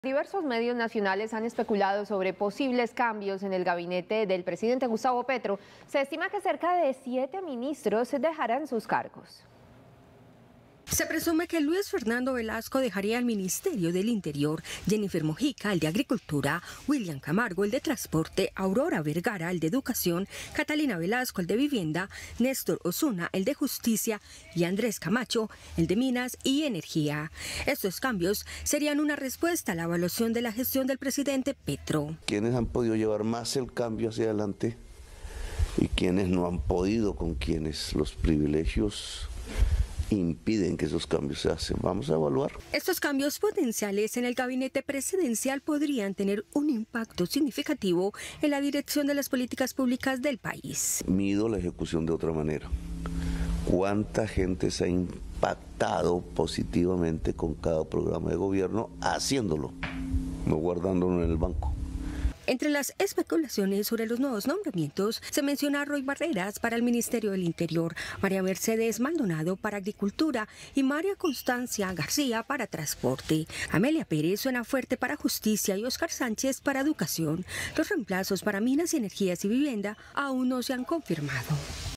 Siete ministros saldrían del gabinete Petro
Diversos medios nacionales han especulado sobre posibles cambios en el gabinete del presidente Gustavo Petro. Se estima que cerca de siete ministros dejarán sus cargos.